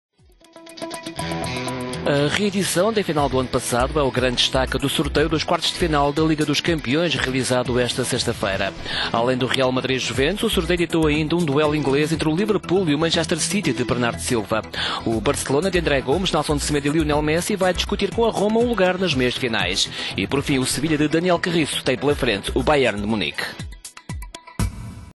Liga dos Campeões: Ronaldo em Turim, Messi em Roma e Bernardo em Liverpool
Sorteio dos "quartos da 'Champions'" decorreu em Nyon, ditando ainda os duelos entre Sevilla e Bayern de Munique, Barcelona e AS Roma…